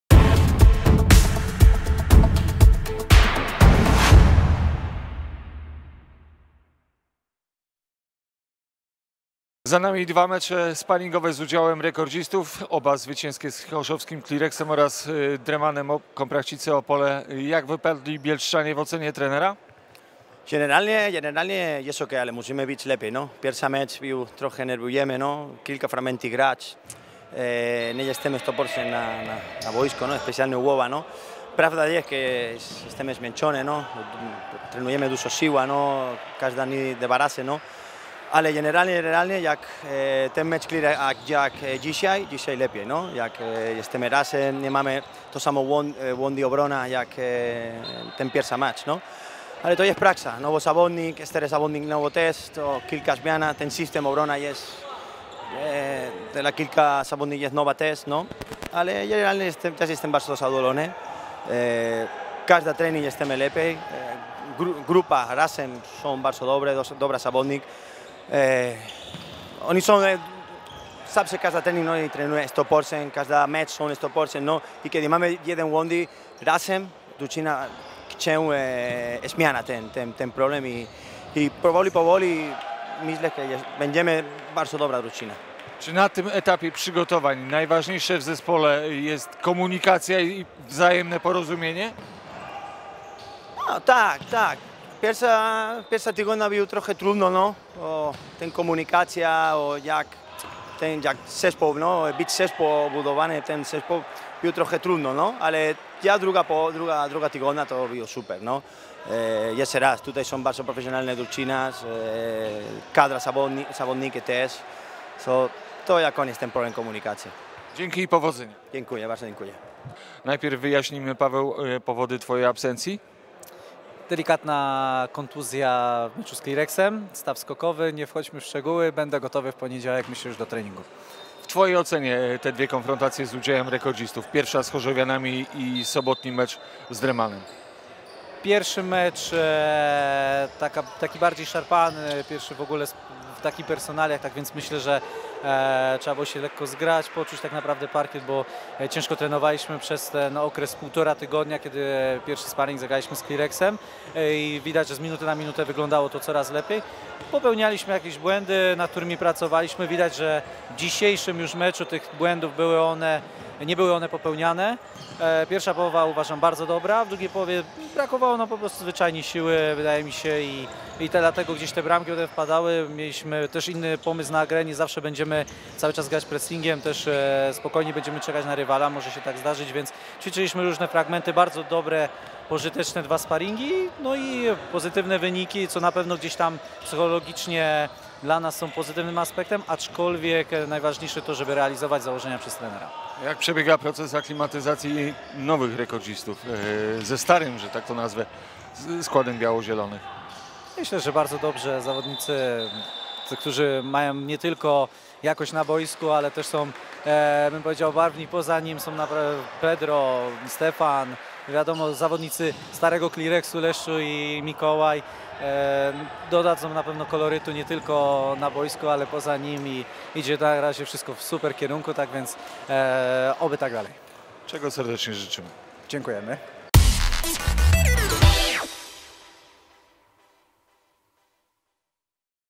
Rekord Bielsko-Biała - Dreman Komprachcice/Opole 6:3 (4:1)
Druga tego lata gra sparingowa naszych futsalowców, duga również wygrana. Tym razem z solidnie wzmocnionym Dremanem Komprachcice/Opole